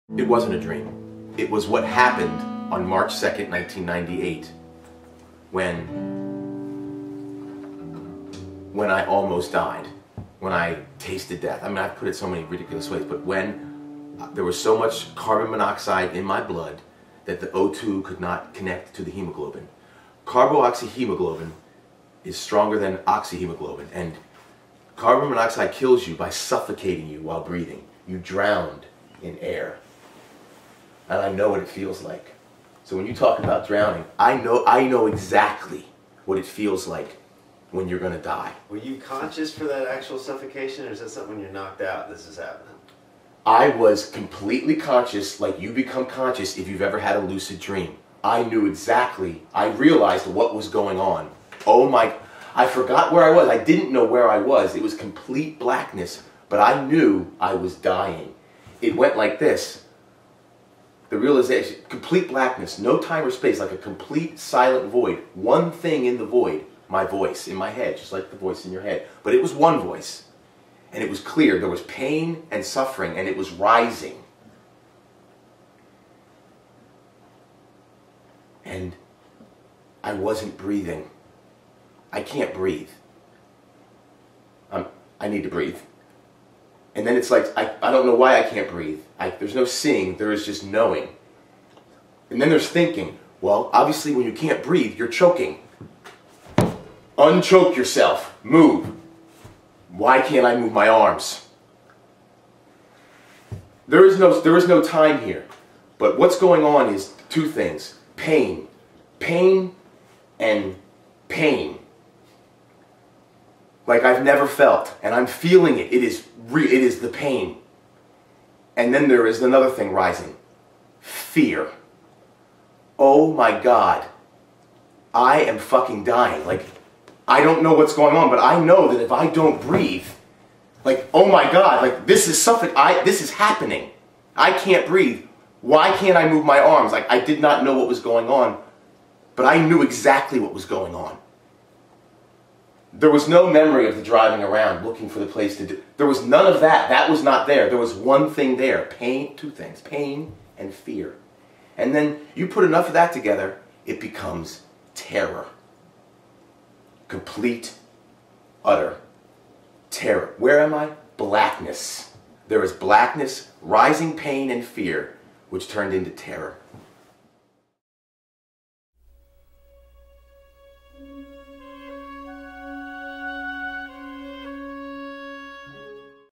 My suicidal near death experience part 1 | frank talk about suicide | episode 2
I would like to acknowledge and appreciate my 'old friend' Estes Shane Whalen, and a 'new friend' Noah Caveny for helping to create this conversation.